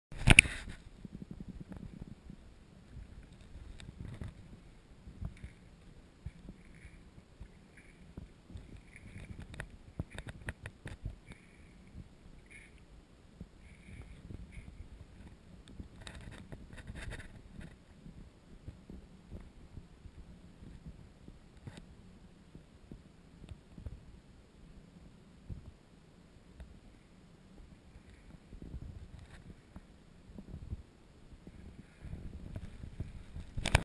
Rough Skinned Newt Breeding Masses
3 mating balls of Rough-Skinned Newt (Taricha granulosa) observed in East Austin Creek, tributary to Austin Creek and the Russian River.  September 2012.